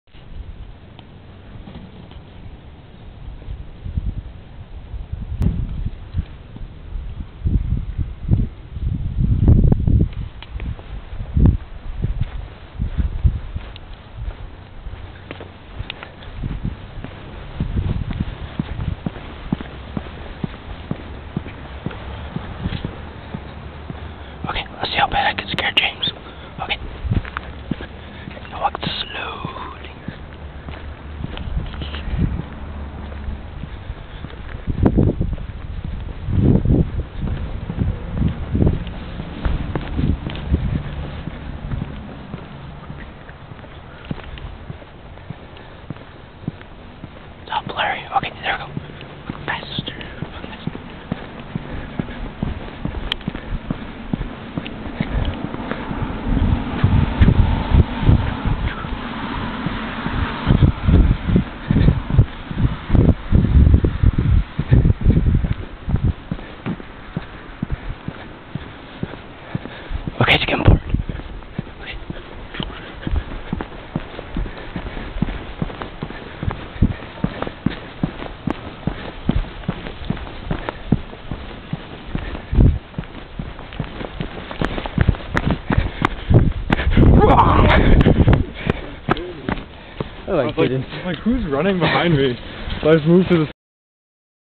Stalking James
I was trying to scare James when I went to meet him a few days ago but I had to wait in the freezing cold 'cause he was 17 minutes late, then I try to catch up to him and he's like speed walkin'!